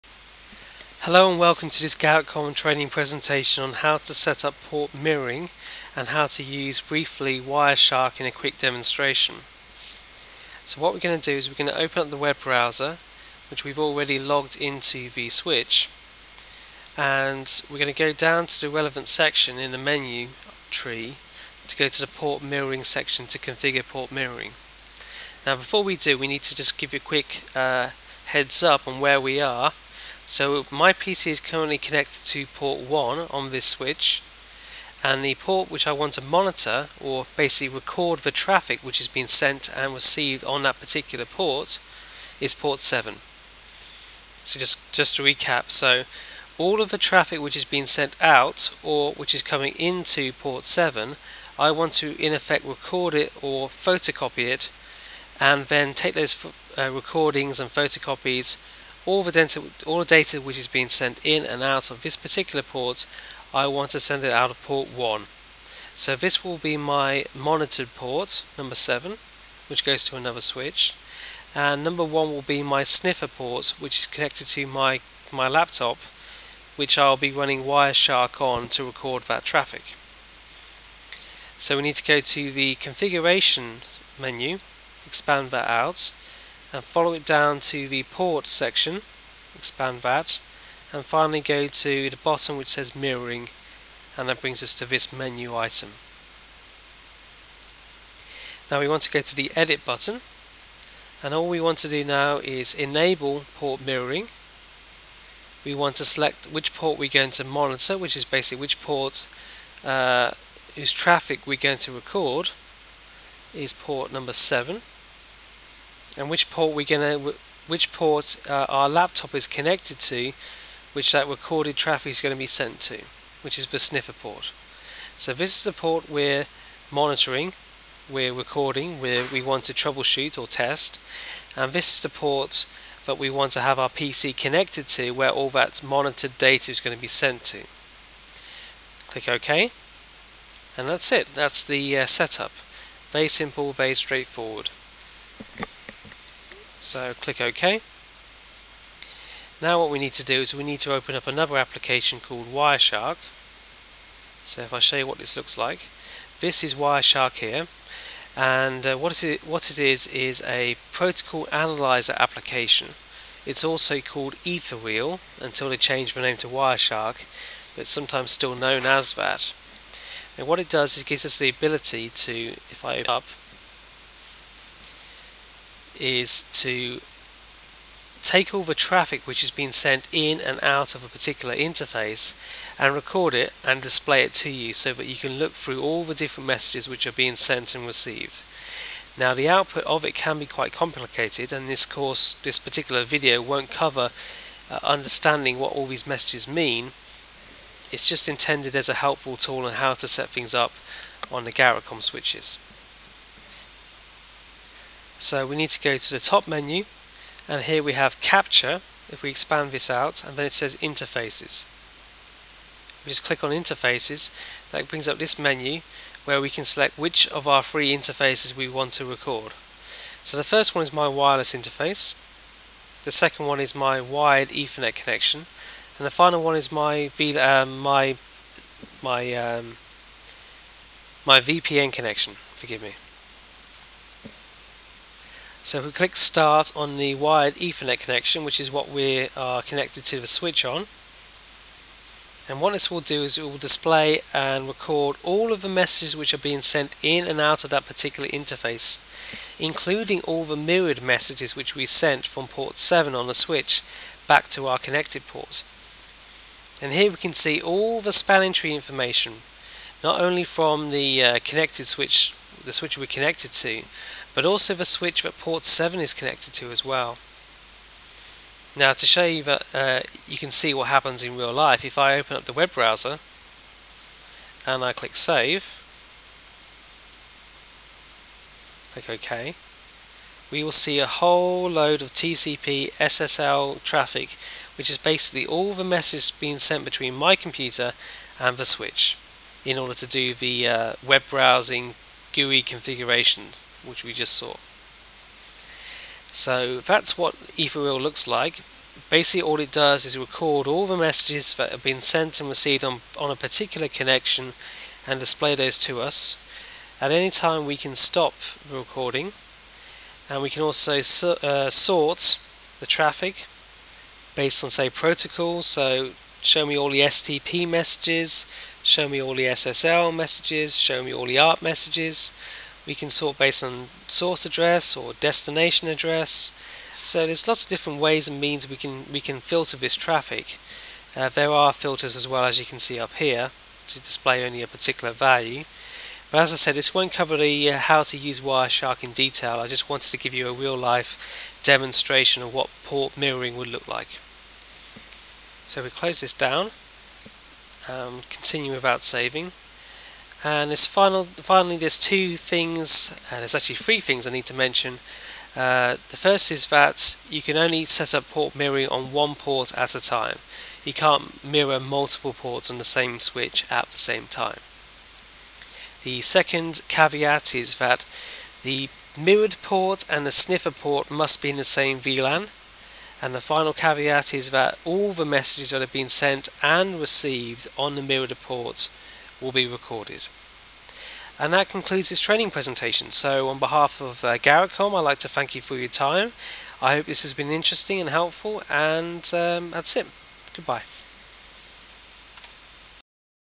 GarrettCom: Port Mirroring Setup and Demo
This GarrettCom training presentation covers how to use the GUI on the 6K managed switch to perform Port Mirroring.  This feature is very helpful for monitoring and troubleshooting purposes.  This video also includes a practical demonstration using Wireshark